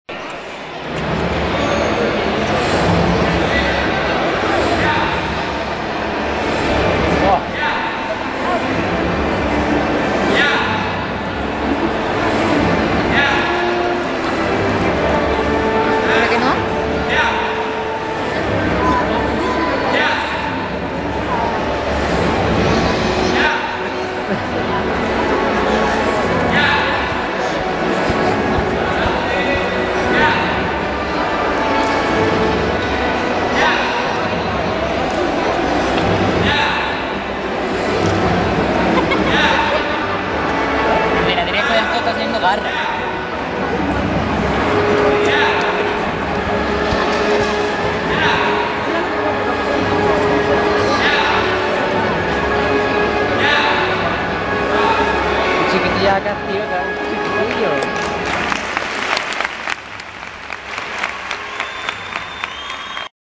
Exhibición de artes marciales en Antequera - 7 de Abril 2013 - Kunfu Mantis
Exhibición de arte marciales en Antequera - 7 de Abril 2013 - en el Polideportivo Fernando Argüelles

  Como ha dado a conocer el concejal de Deportes, Alberto Vallespín, desde las 11:00 horas, los asistentes podrán disfrutar de un "cartel inmejorable" con varios maestros de Antequera y parte de la provincia en disciplinas como Kung Fu, Kick Boxing, Kárate, Chow-Gar, Jansu Do, Mawari-Shiwari, Ninjutsu, Chi Kung, Budo Taijutsu, Wu Wei Do, Taekwondo, Jiu Jitsu, Hapkido o Krav Maga.